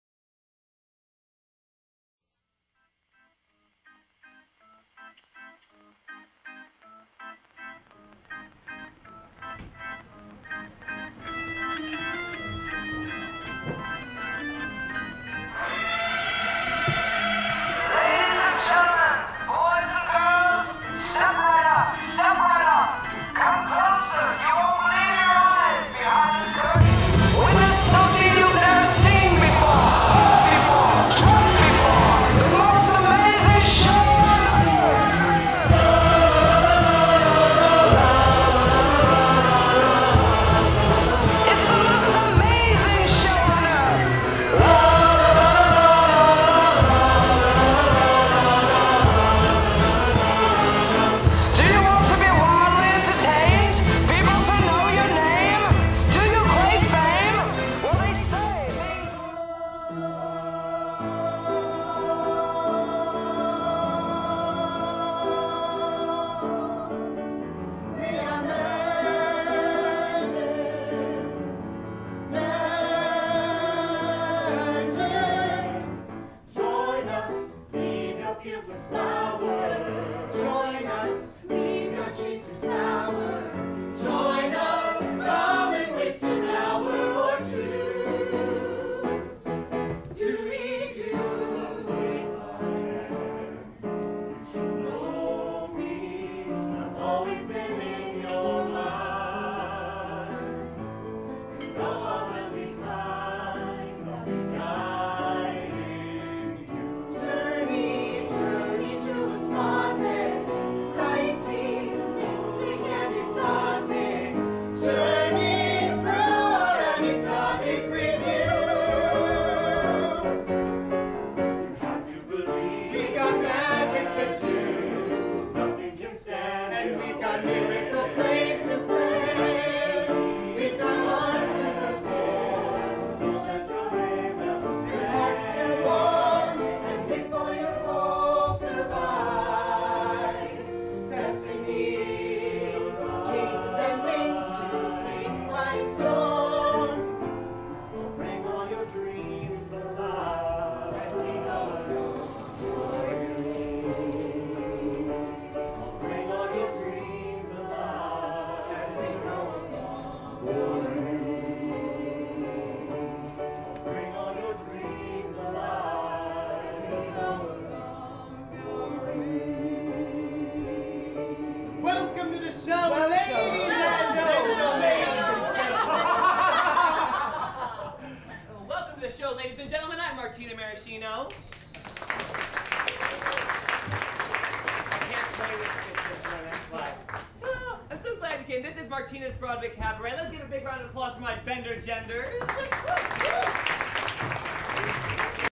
Magic Mix-Up
Magic Mix-Up performed at Martina's Broadway Cabaret: Bender-Gending/Produced by Silver Slipper Productions/Bryant Lake Bowl Theatre/Minneapolis, MN 2013

Eric F Avery
Evan Boyce
Lindsey Brown
Jesse Frankson
Shannon Foy
Seana Grace
Robbin Molnar
Lauren Traynor
Seth Gabriel as Gabe Gabriel
and
Martino Mayotte as Martina Marraccino